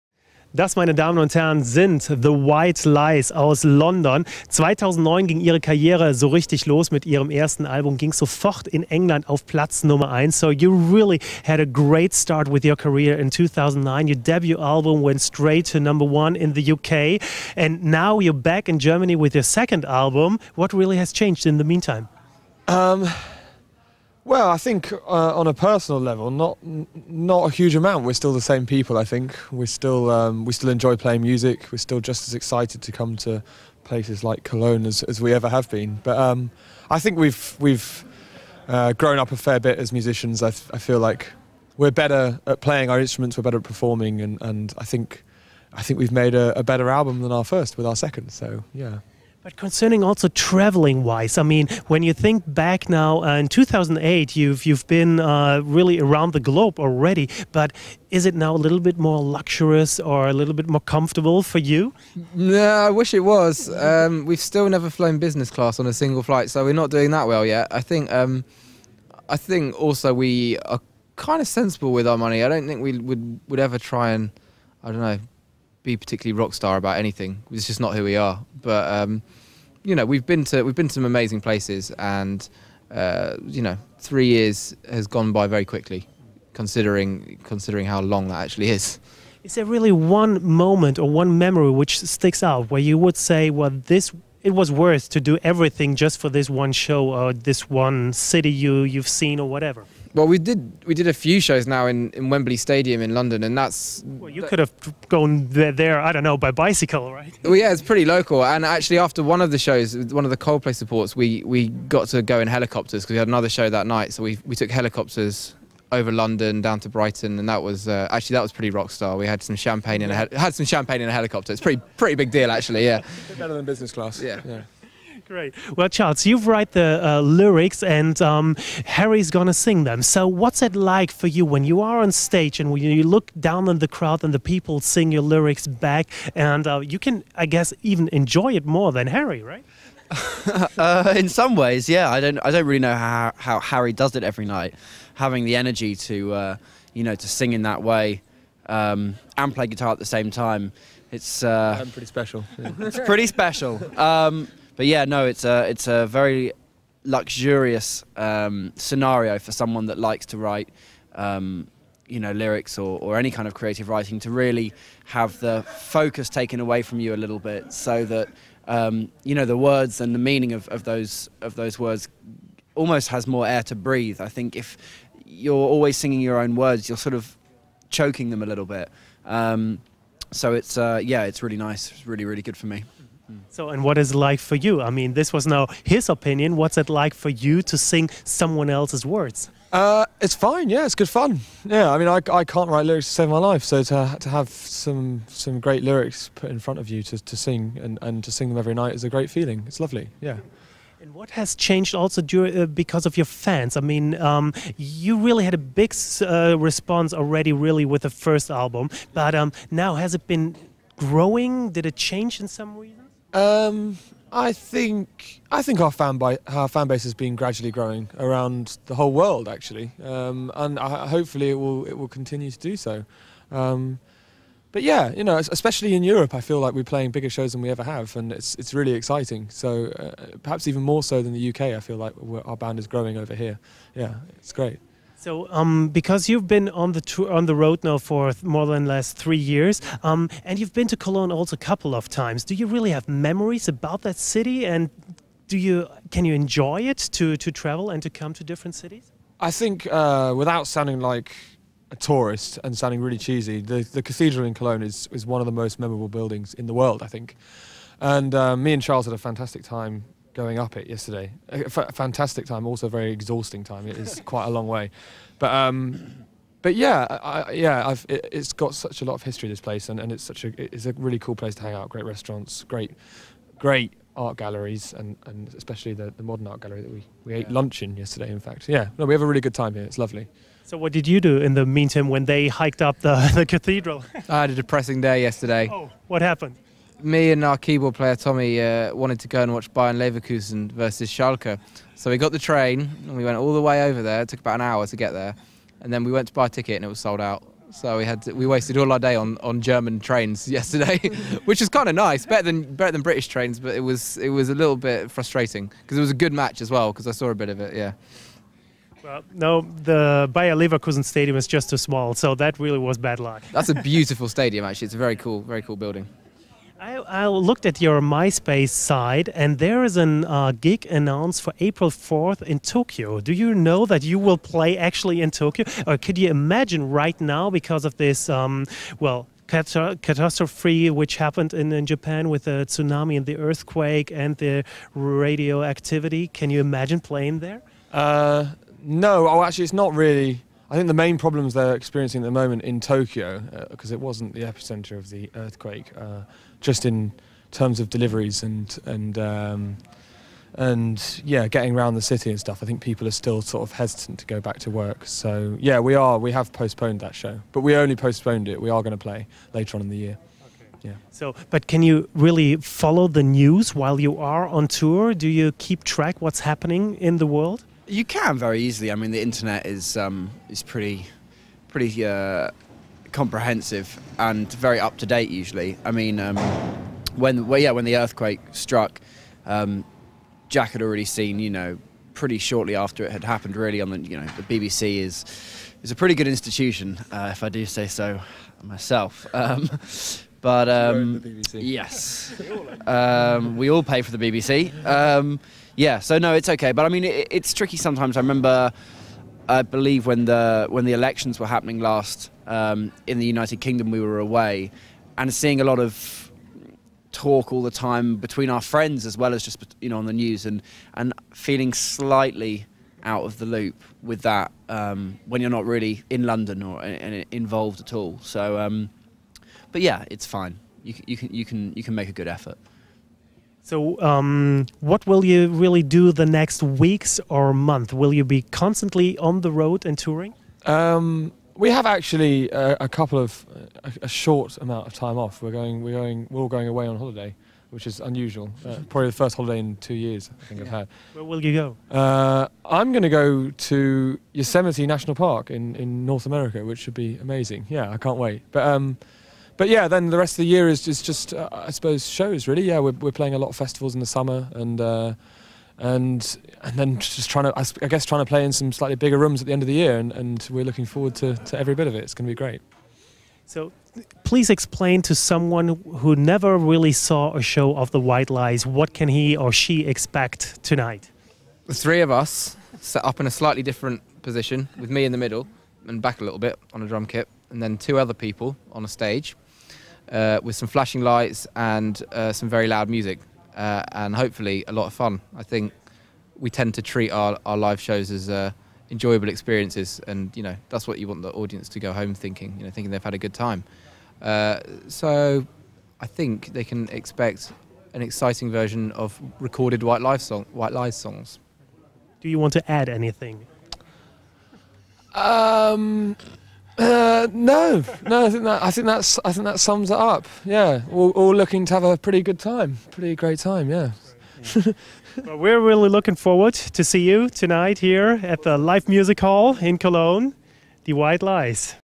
WHITE LIES- INTERVIEW AT ROCKPALAST 2011
WhIte Lies answering questions like  how Charlie feels about Harry singing his lyrics ,why Jack spent a whole day in German trains and why they called off their Asia Gigs